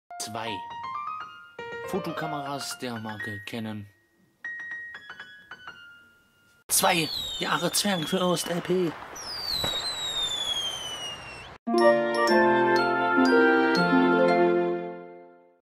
Zwei Jahre 🎉
zweijähriges Jubiläum von Zwergenfürst LP

Vorm Erfolg, wie jeder weiß
Fließt ja immer sehr viel Schweiß
Eifrig habt ihr viel geschafft
Glück, Gesundheit und auch Kraft
Sollen weiter euch begleiten
Wie sich ändern auch die Zeiten

Meine Fr***e! Es hat länger gedauert dieses dämliche Glückwunschgedicht rauszusuchen, was absolut nicht zu diesem Anlass passt (Quelle als es gedauert hat dieses wahnsinnig aufwändige Video zu schneiden, zu rendern & hochzuladen. In der Zeit, in der ich diesen Text hier tippe, hätt ich wahrscheinlich ungefähr noch das Video für die 3 Jahres- und die 4 Jahres Party vorproduzieren können. S' liest ja eh niemand bei so nem kurzen Video die Videobeschreibung. Aber was soll's.
Auf zwei weitere wundervolle Jahre Zwergenfürst LP!